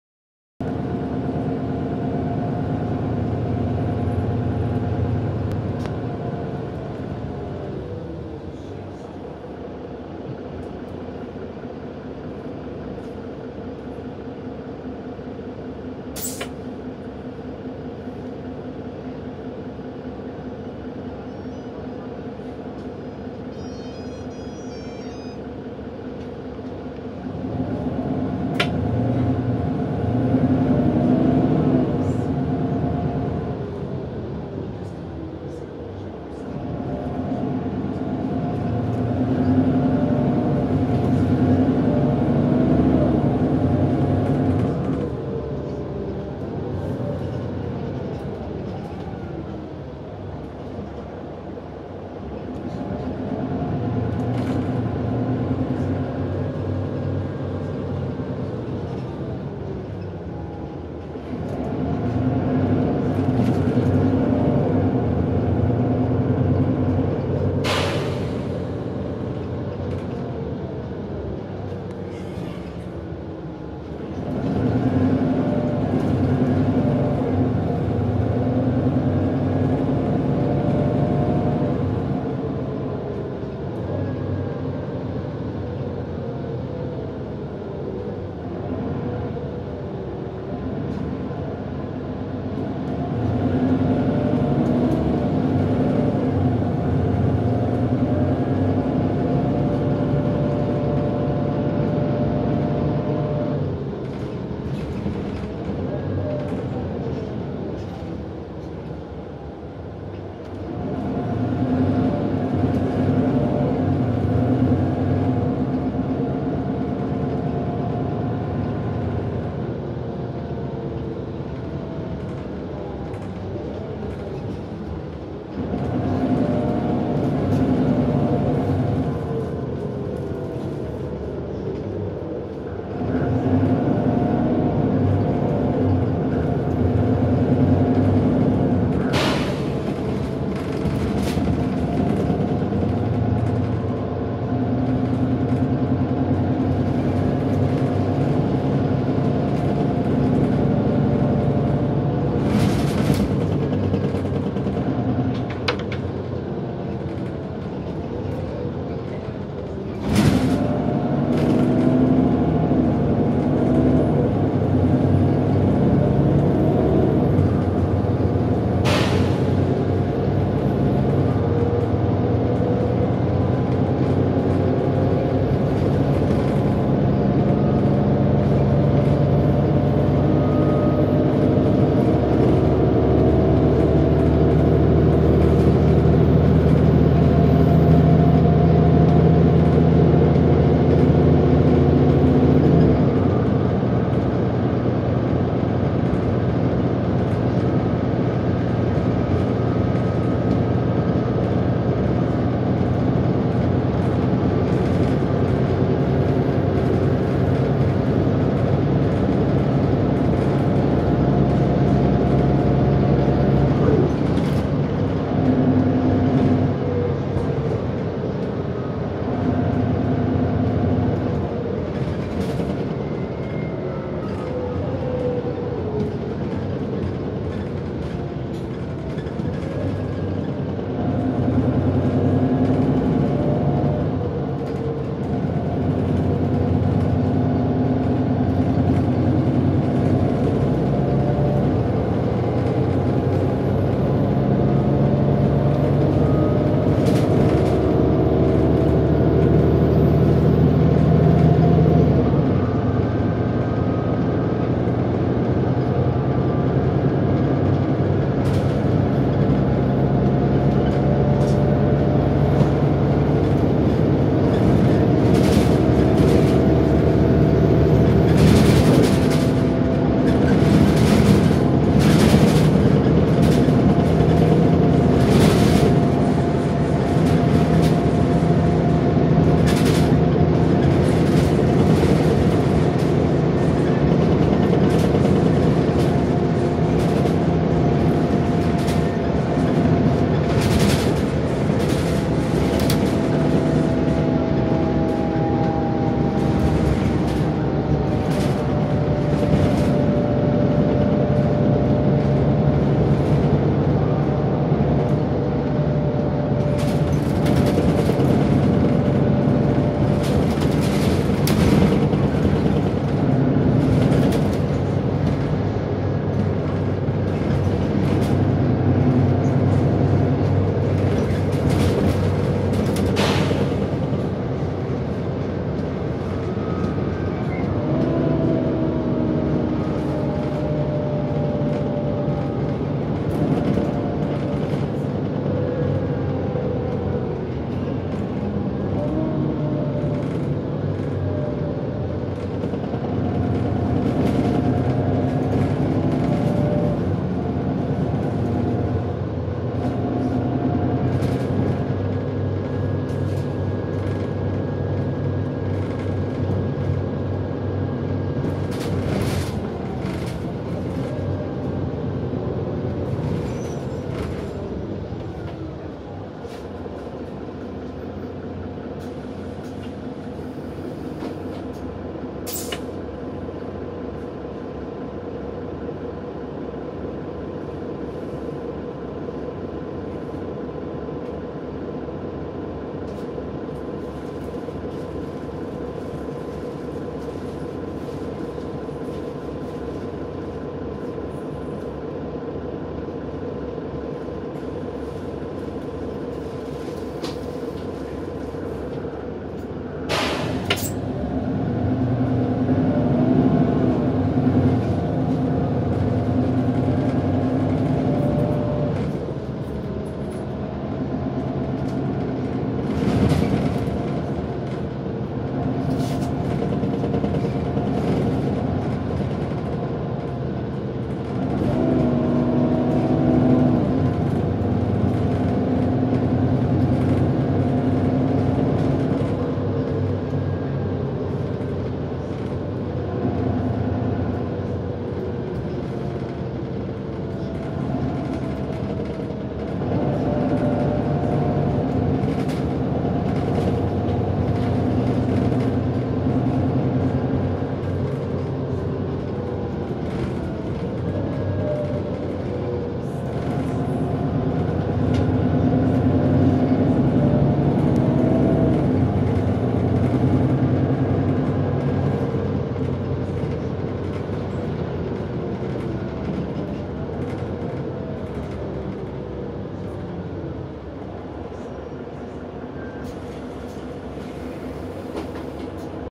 Arriva Max Across The Shires Mercedes Benz Citaro 3908 BU06HSN. Route 150 Milton Keynes To Aylesbury
Arriva Max Across The Shires Route 150 Branded Mercedes Benz Citaro on Its Route To Aylesbury From Milton Keynes Via Leighton Buzzard.
Aylesbury Depot Has 3 Max Routes:
Route 150 Aylesbury To Milton Keynes Via Leighton Buzzard
Route X60 Aylesbury To Milton Keynes Via Buckingham
Route 300 Aylesbury To High Wycombe Via Princes Risborough.
Bus Route: 150
Milton Keynes Station - The Point - MK Stadium - Heath & Reach - Leighton Buzzard - Wing - Bierton - Aylesbury
Bus Journey: From Leighton Buzzard To Wing
Bus Number: 3908
Bus Registration: BU06 HSN
Bus Depot: Aylesbury